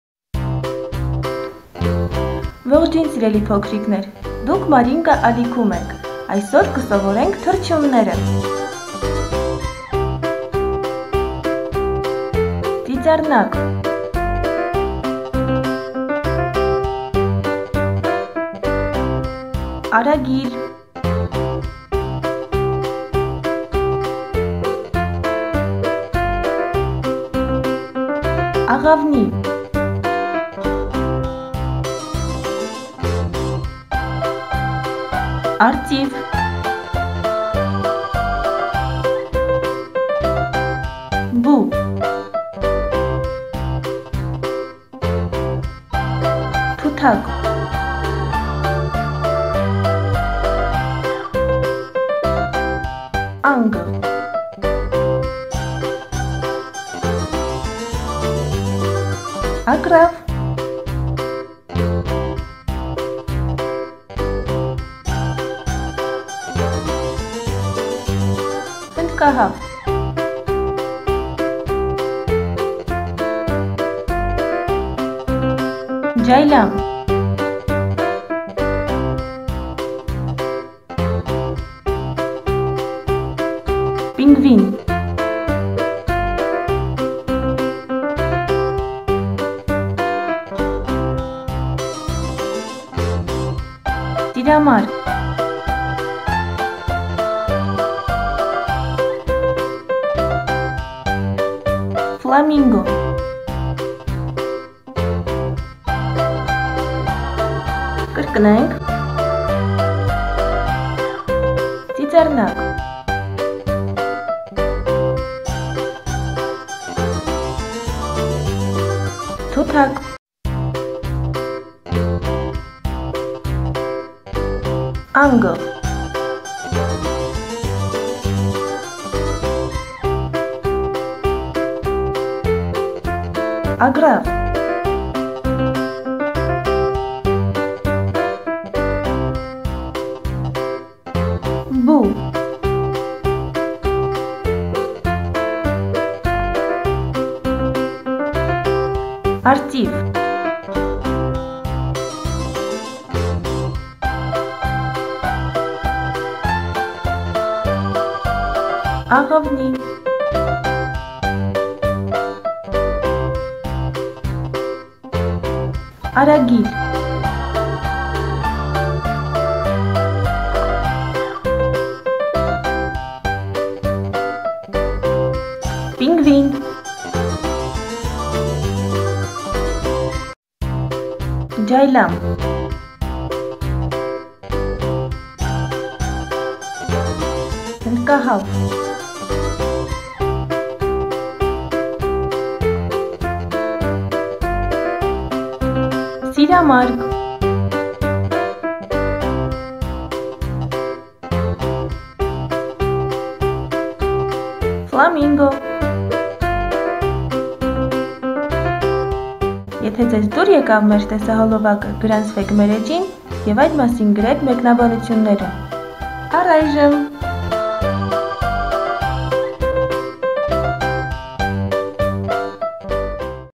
Սովորում ենք թռչունները / Educational video about birds for kids / Учим птиц по армянски
Educational video about birds for kids
Zargacnox xaxer . sovorum enq trchunner
Զարգացնող խաղեր . սովորում ենք թռչուններ
Развивающие и обучающая видео про птиц для самих маленьких
Игры птицы для детей 2-5 лет